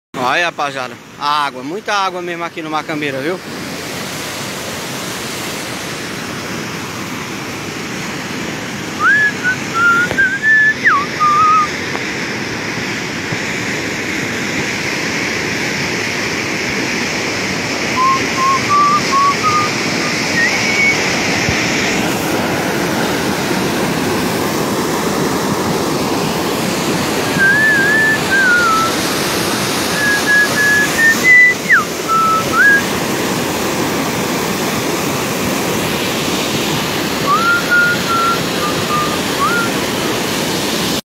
Mais uma barragem sangrando no Ceará
Barragem do Cascavel, águas do rio Macambira. Na região da Macambira, Poranga-ce, tem encantos. GRATIDÃO a Deus pelo ano bom de inverno. Vídeo foi gravado 10 de Março de 2020   Geovani primo do poeta Rodolfo Bezerra.  Show